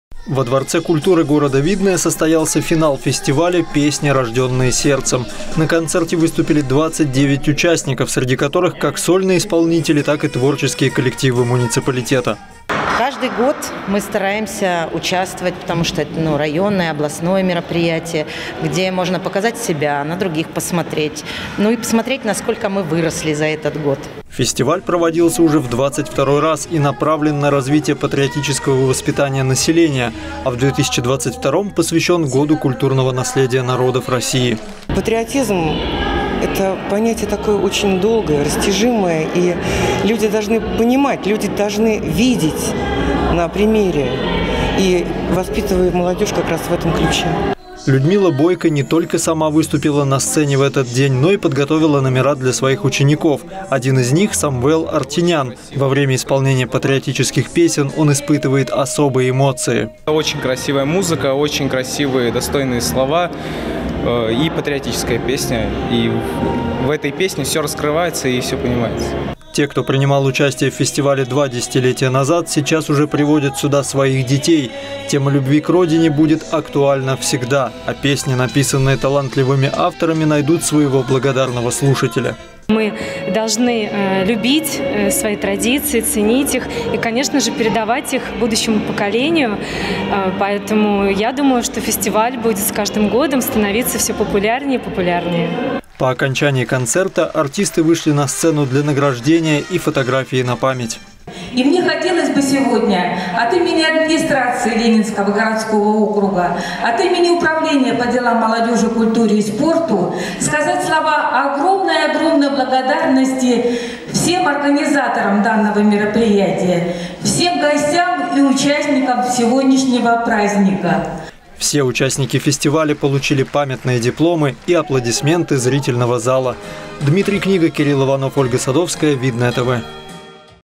ПЕСНИ, РОЖДЁННЫЕ СЕРДЦЕМ
Фестиваль патриотической песни "Песни, рождённые сердцем" - традиционное мероприятие, ежегодно проводимое в Ленинском городском округе. Сначала проходят отборочные этапы, после чего лучшие артисты выступают на финальном концерте.